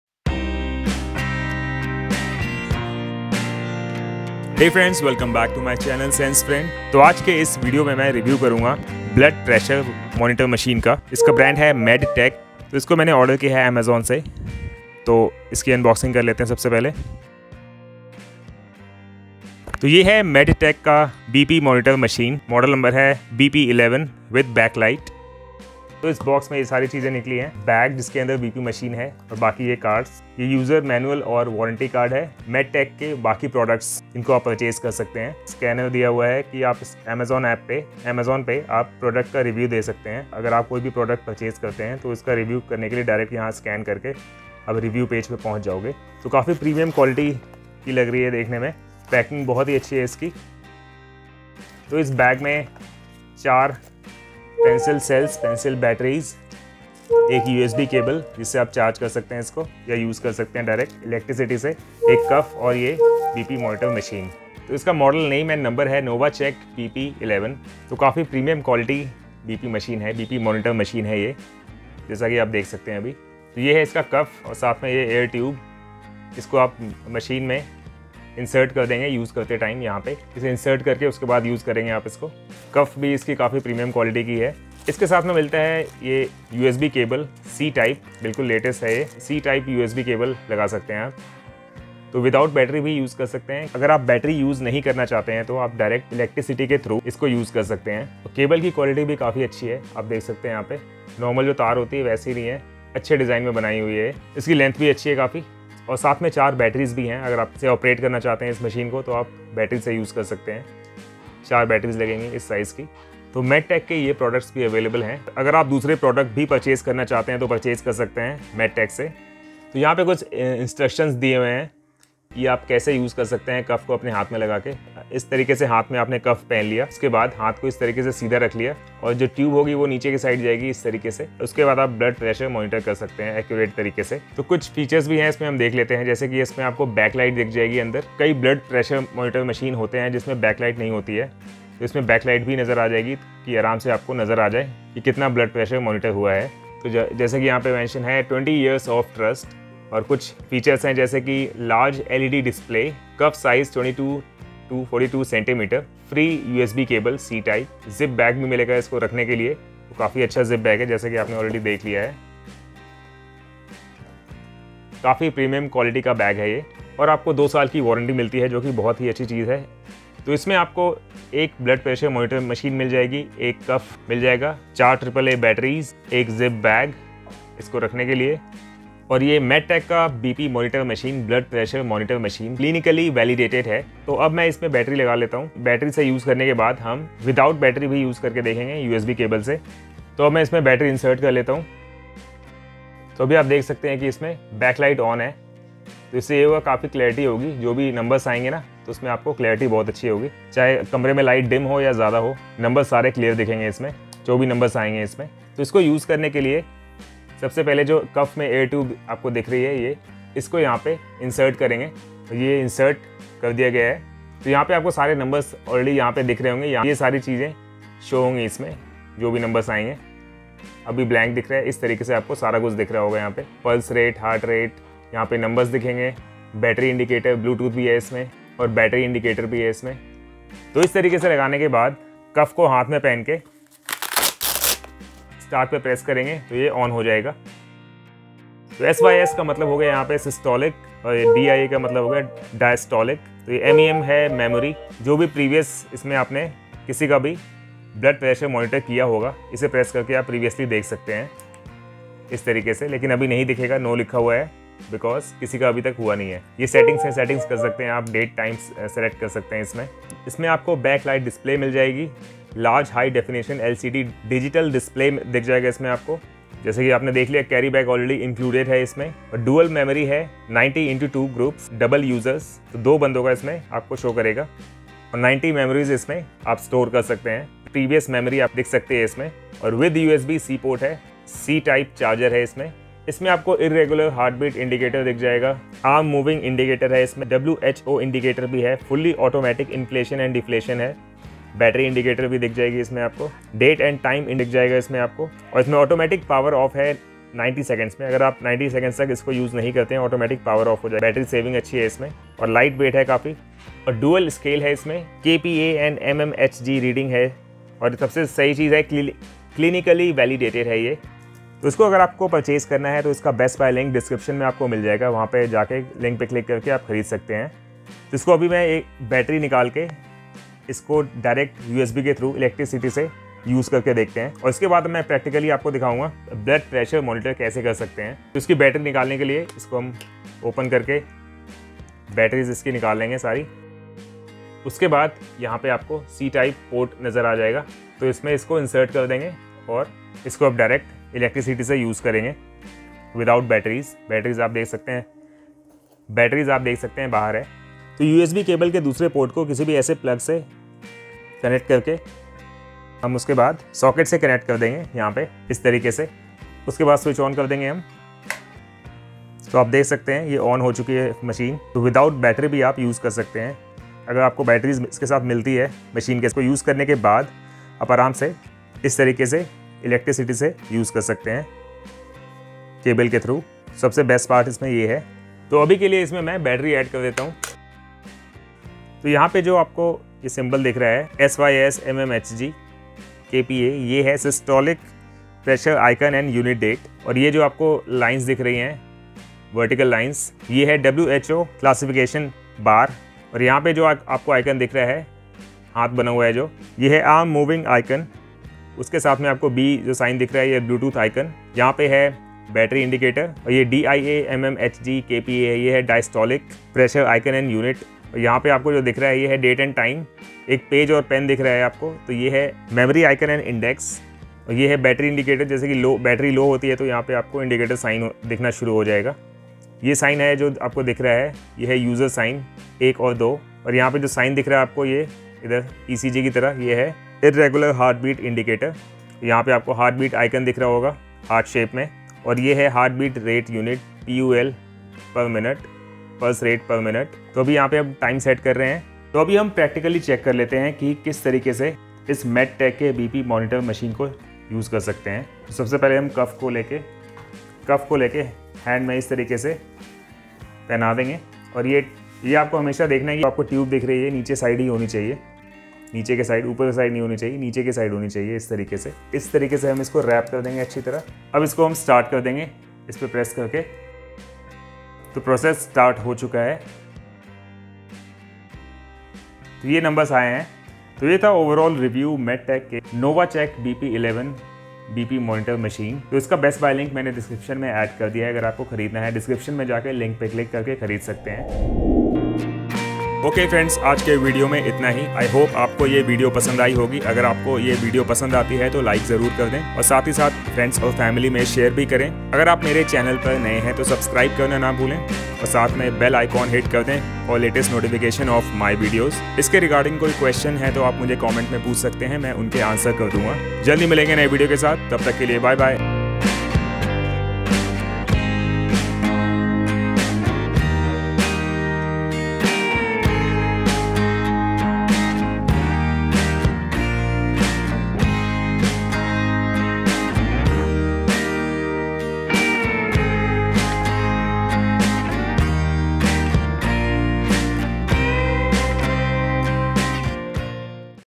MEDTECH Digital Blood Pressure BP Monitoring Machine Smart MDD Technology | DEMO: How to use it ?
Best Buy Link BP Machine Medtech:
Medtech BP11 BL Portable Automatic Digital Blood Pressure BP Monitoring Machine with Smart MDD Technology

In this video I will provide you detailed information about BP Monitor Machine Premium Quality | MEDTECH Blood Pressure Machine Review & How to use it ? | Demo.